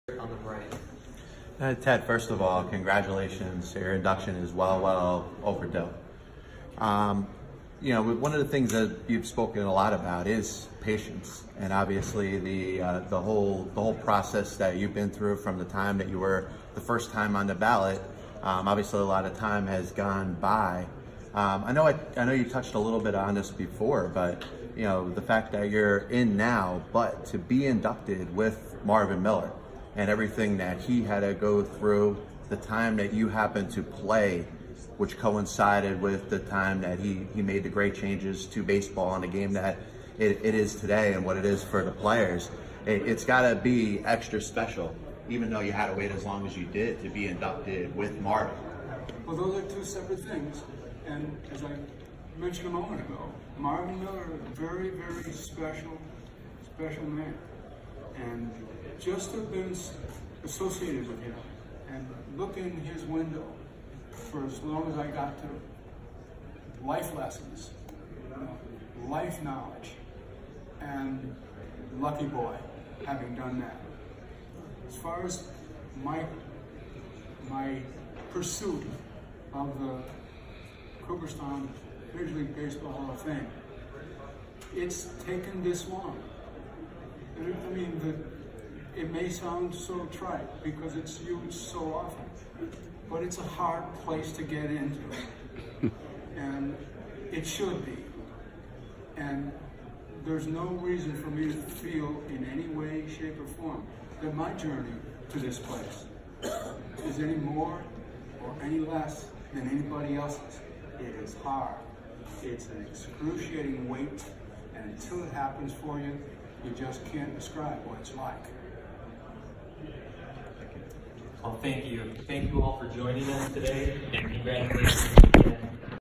12/09/2019 Passed Ball Show- John’s question to Baseball Hall of Famer Ted Simmons
12/09/2019 Takes place at the Baseball Winter Meetings in San Diego, CA. At the press conference to celebrate the long awaited selection of new Baseball Hall of Fame Catcher Ted Simmons, John asks a question about the amount of time Ted had to wait to get into the Hall of Fame. Originally dropped from the ballot for not getting enough votes his first time on the ballot, Ted’s case has been made by sabermetrics and the Veterans Committee. Was it any more special because he got selected with Marvin Miller? Perhaps that made up for some of the wait.